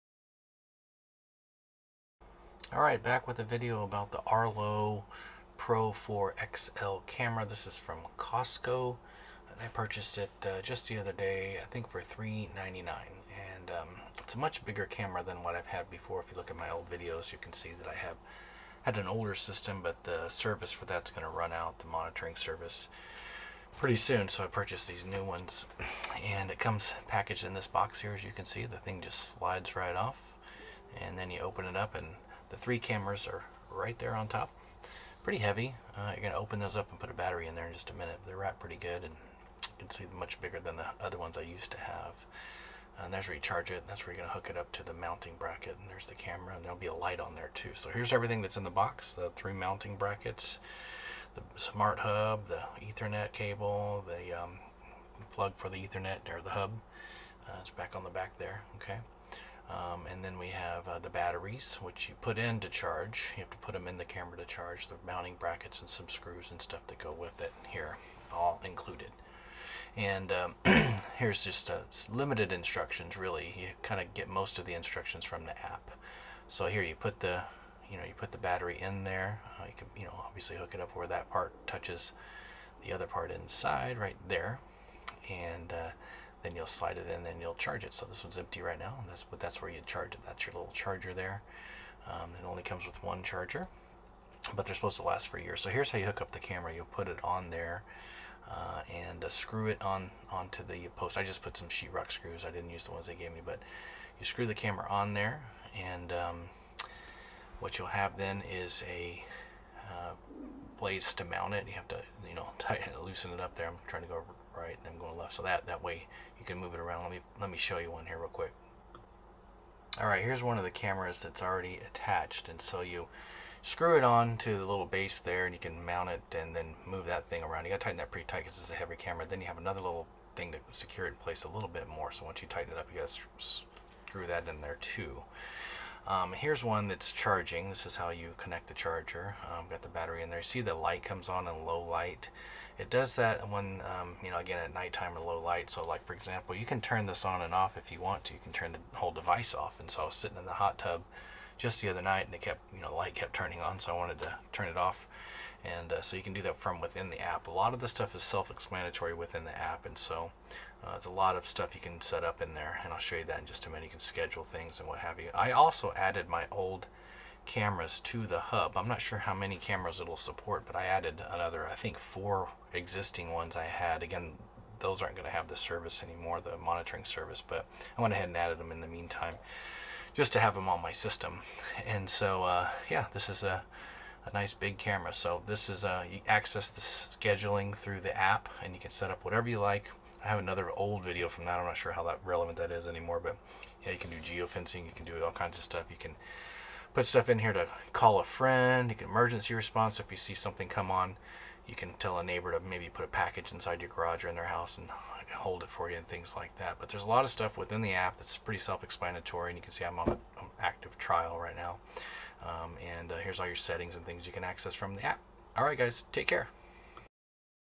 Arlo Pro 4 XL Spotlight Camera System
Unbox and quick demo of Arlo Pro 4 XL Spotlight Camera System.